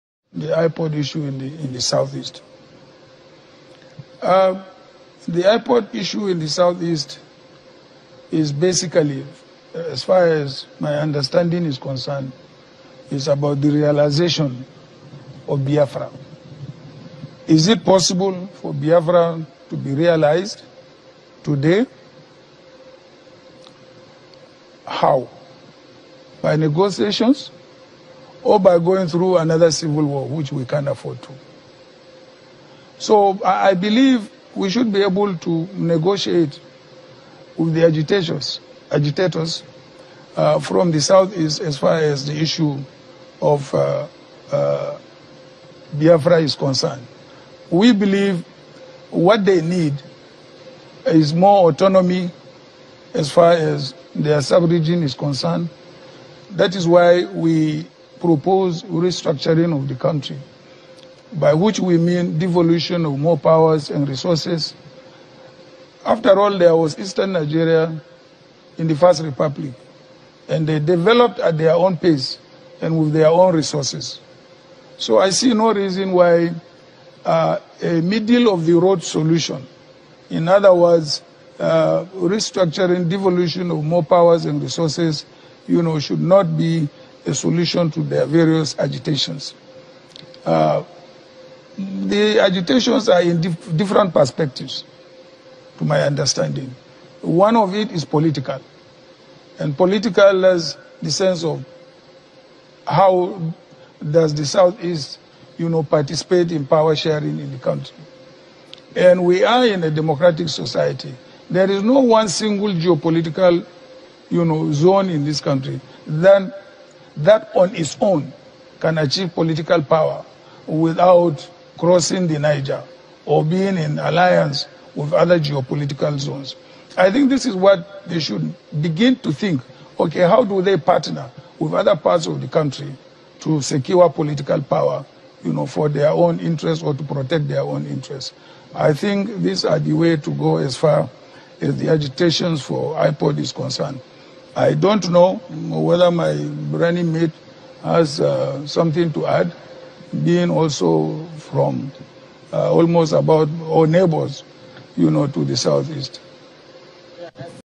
Listen to what Atiku said about Biafra and IPOB Agitation - (Full Video)
Listen as Atiku talked about IPOB and Biafra Agitation.